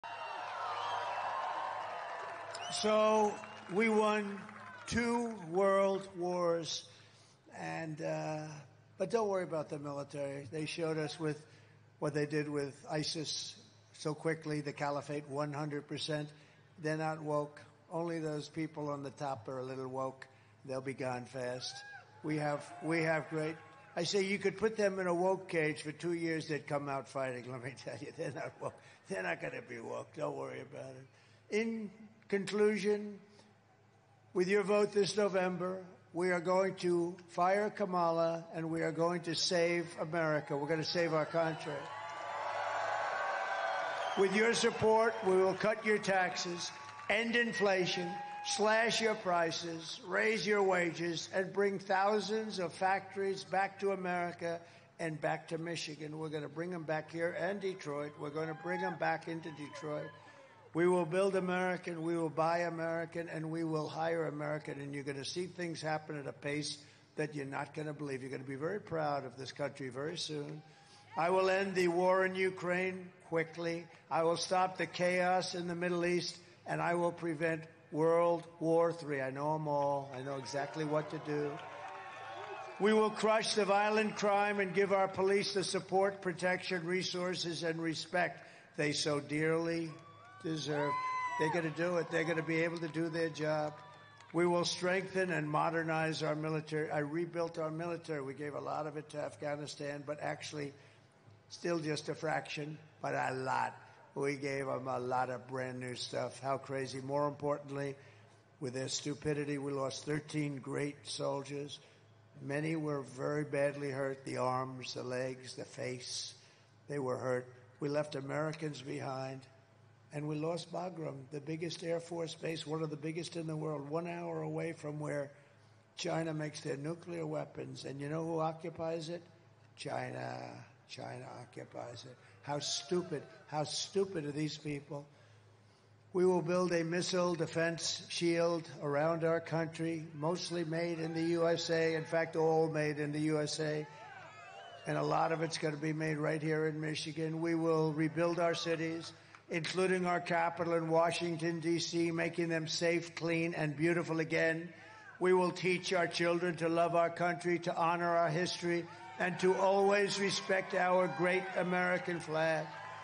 'We Are Going To Save America': Trump Makes His Pitch To Voters In Michigan
At his campaign event Friday night in Traverse City, Michigan, former President Trump made the case for re-electing him in 2024.